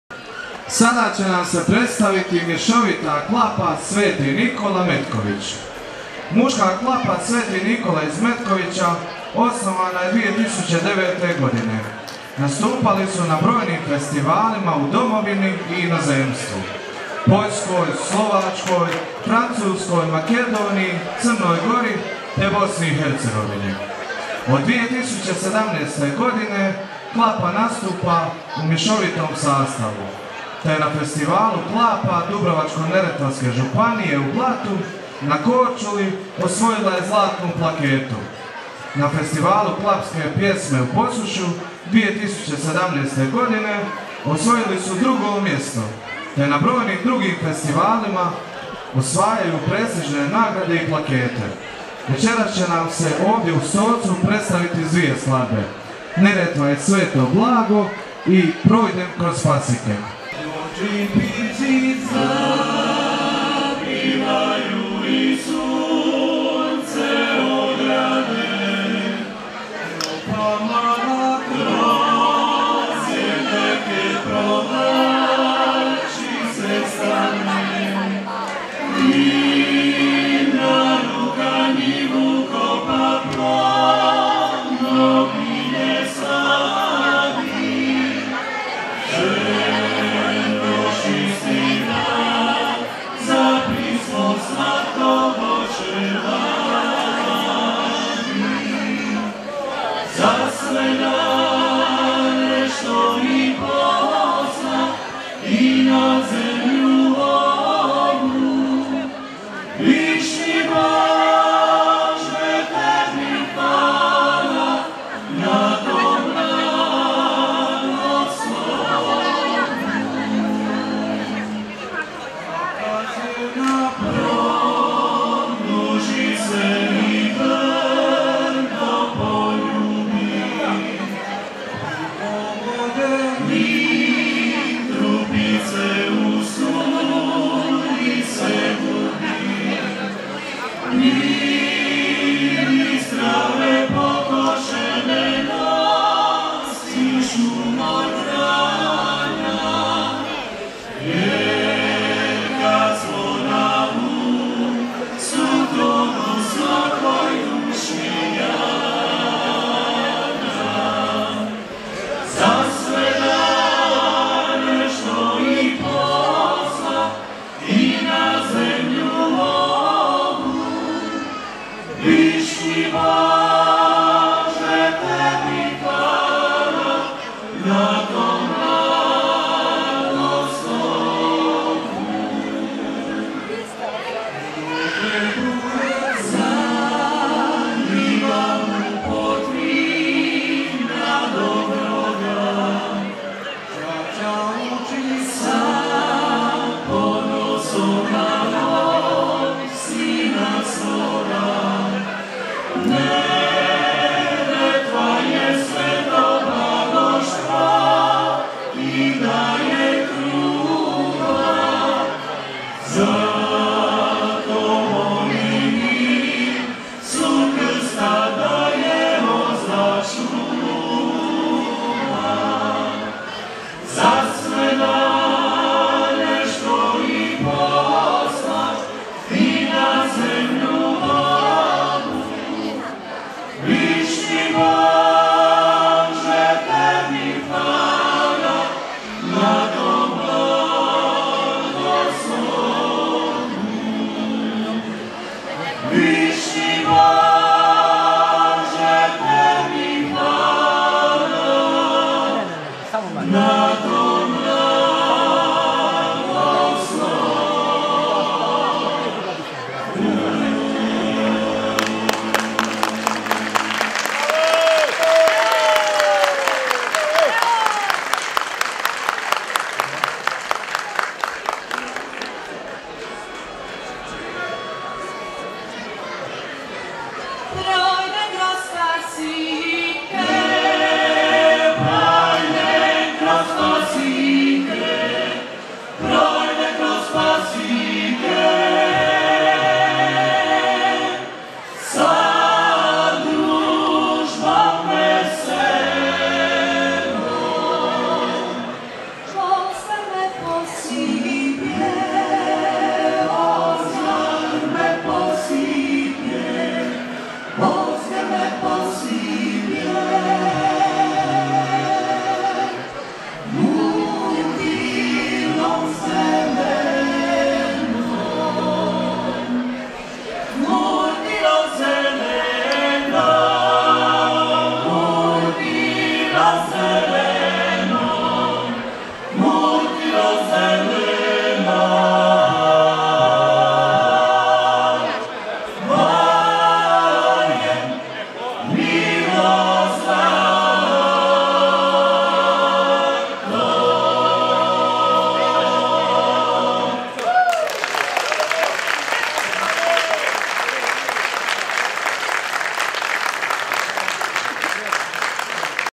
Mješovita Klapa „Sv Nikola“ Metković {19. Večer folklora - Ilindan 2021. Stolac}
U subotu, 17. srpnja 2021. u 20,00 s. održana  je „19. Večer folklora“ u Stocu na vanjskoj pozornici u crkvenom dvorištu.